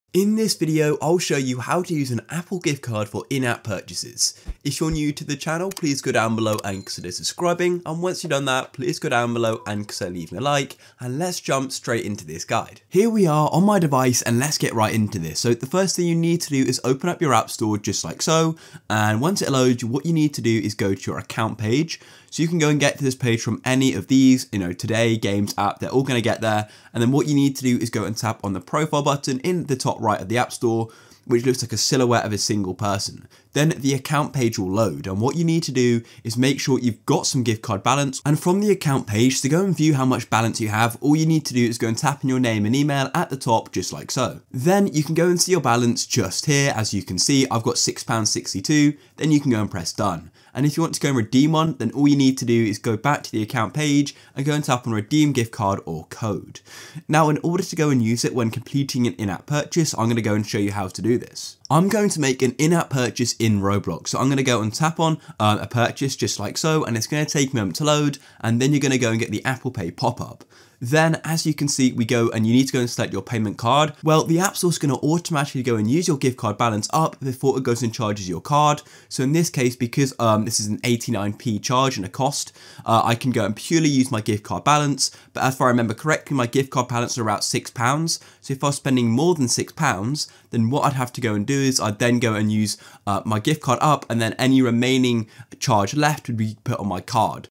How To Use Apple Gift Card For In App Purchases - Full Guide
I show you how to use apple gift card for in app purchases and how to pay for in app purchases with apple gift cards in this video.

GuideRealm